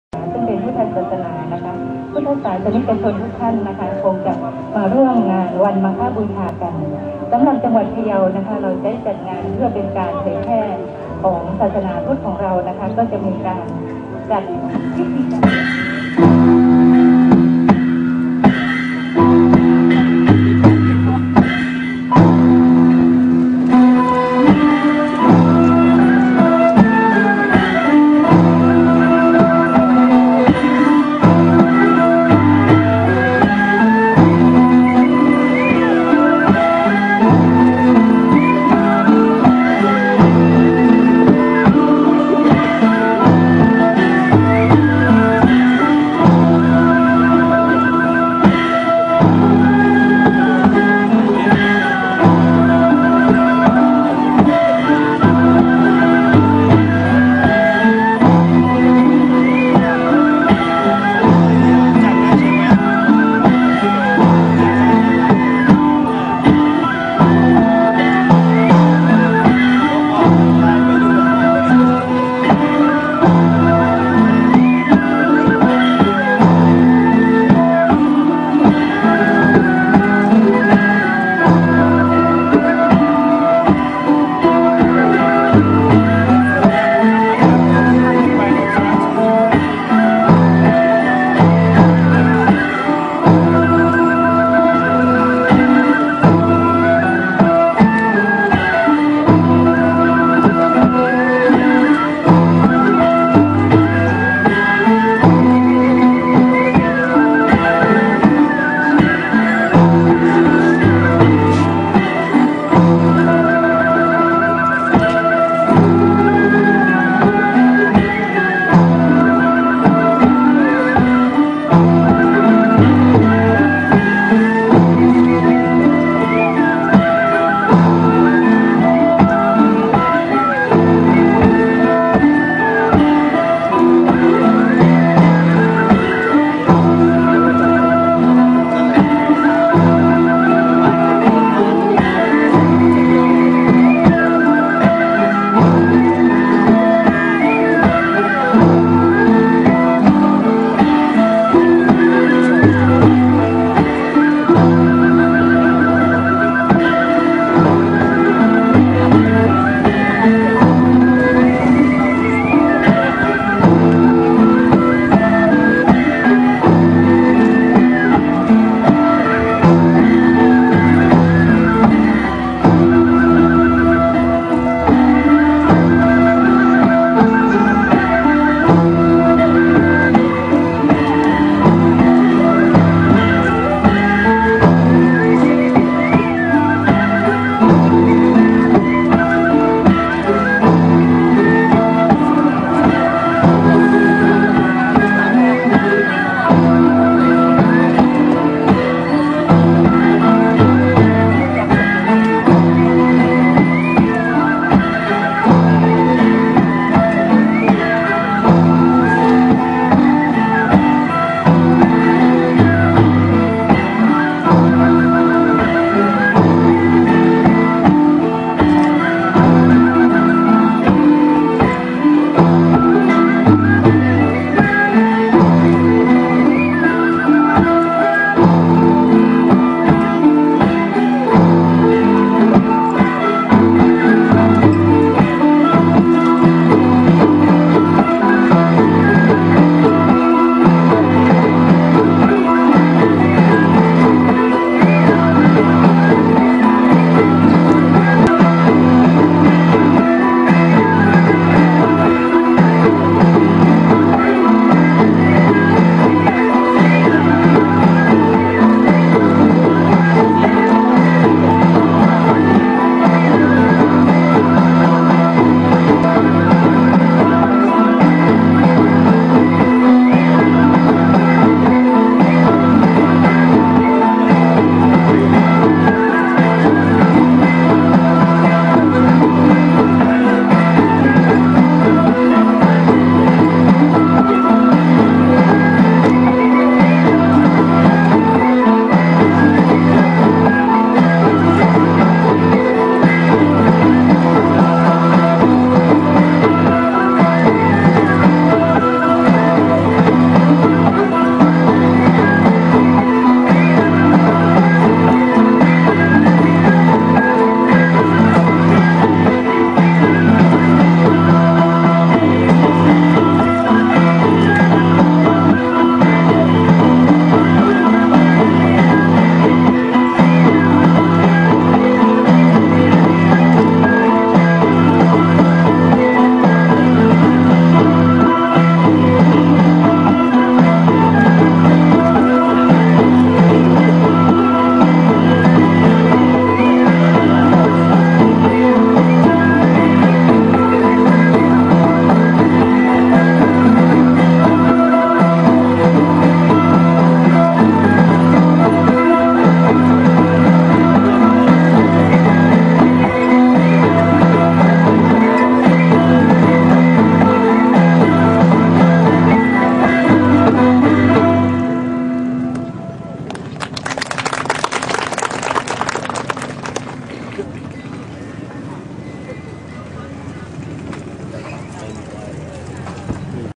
สีสันต์กว๊านพะเยา
สีสันต์และลีลาศิลปะ วัฒนธรรมของกว๊านพะเยาในงาน OTOPบวงสรวงพ่อขุนงำเมือง  ริมกว๊านพะเยา จ.พะเยา(๖มีค.๒๕๕๕)